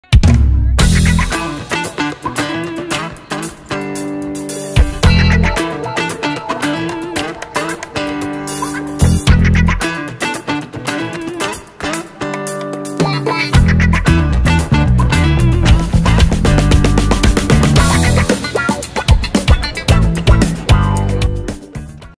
White Bear Lake Catalytic Converter License Stenciling Event
With the sharp increase in catalytic converter thefts, the City of White Bear Lake will host a license plate stenciling event Saturday, August 28 at WBL Public Works. And it's free!